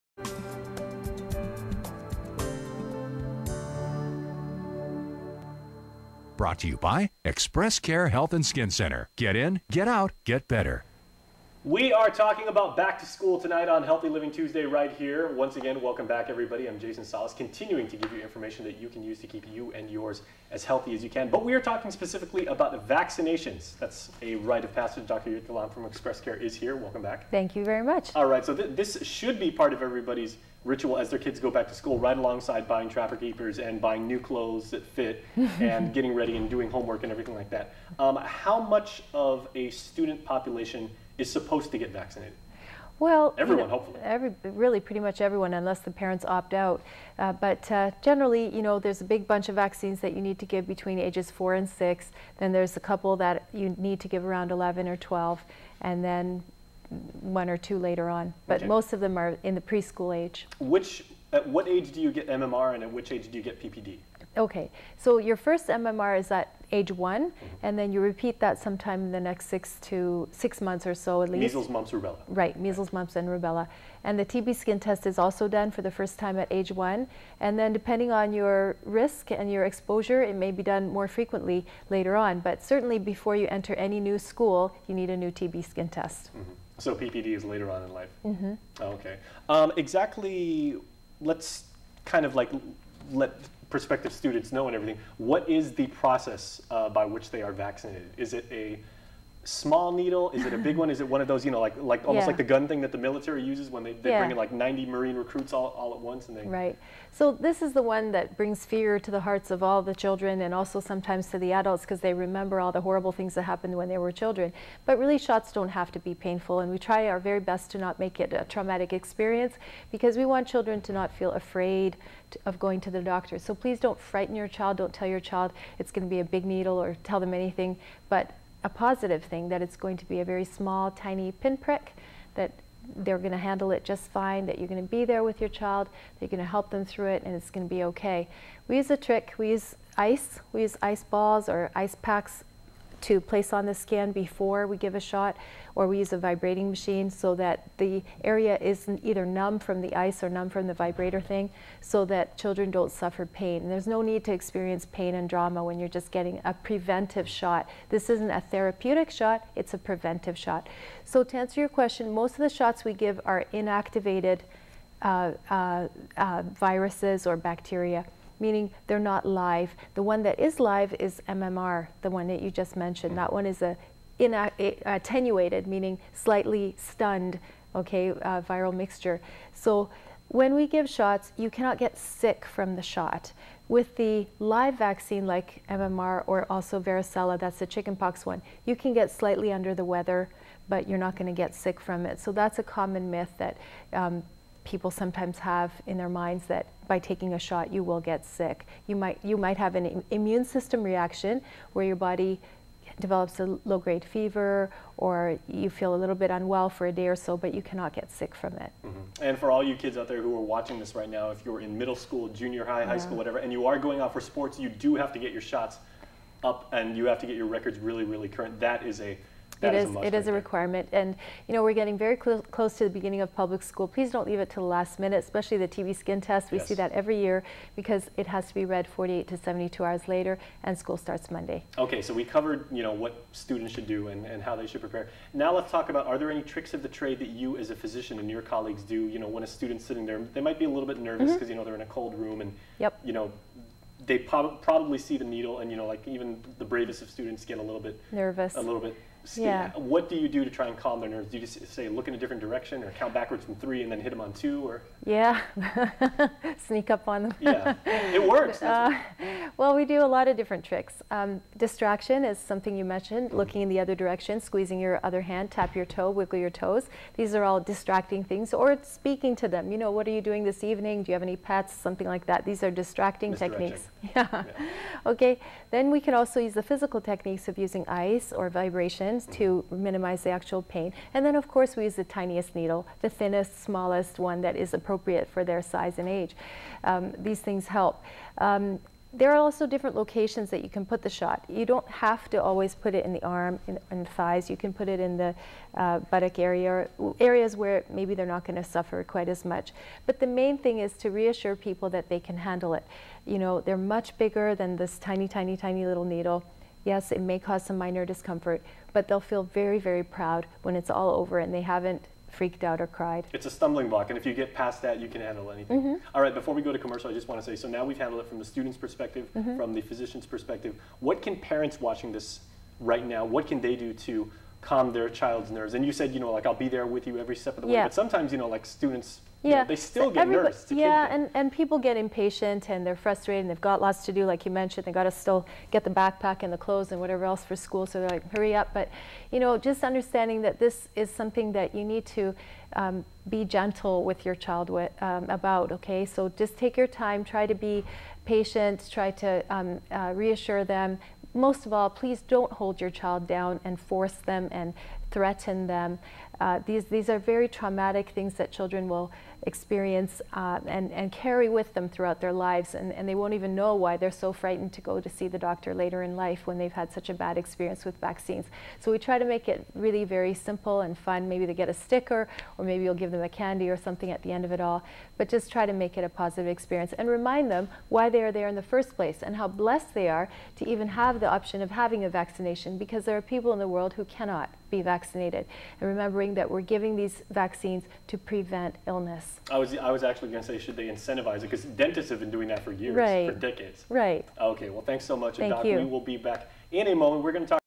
Healthy Living with Dr. Jitka Lom: VACCINATIONS (1 of 2)
Dr. Jitka Lom with ExpressCare shares expert advice on medicine and healthy island living.